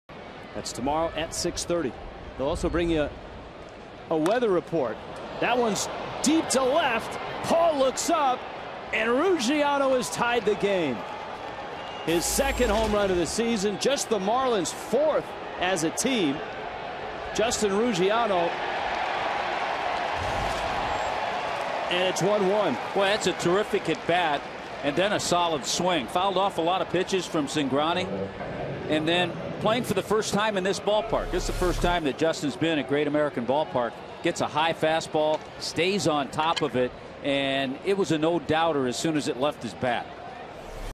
2013/04/18 Ruggiano's solo homer
MIA@CIN: Ruggiano ties the game with solo shot